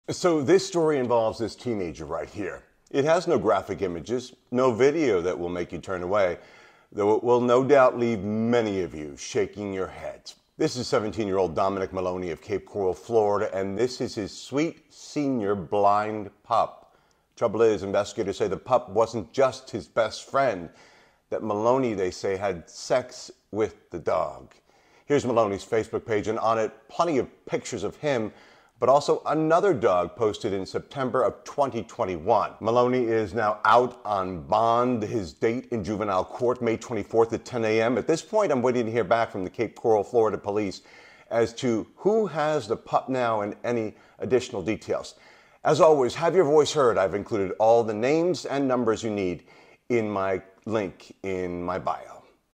Cops: Teen violates sweet senior pup
Cape Coral, Florida teen accused of violating sweet senior pup

Office of the State Attorney Amira Fox